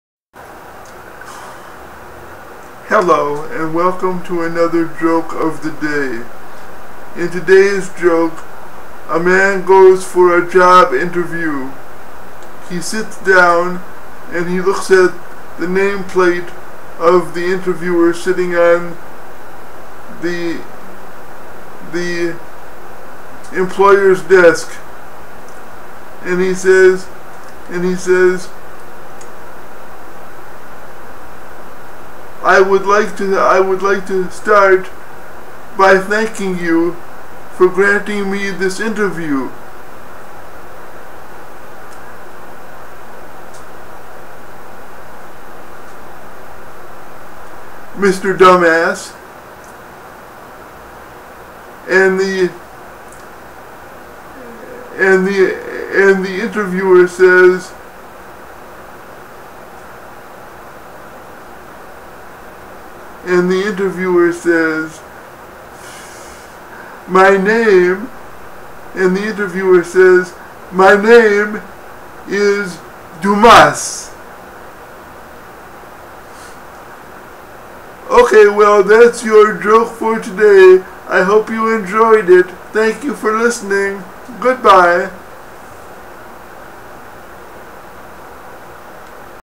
The shortest job interview ever joke
Years ago, I saw this joke in a TV commercial, and for some reason, this morning I woke up laughing about it.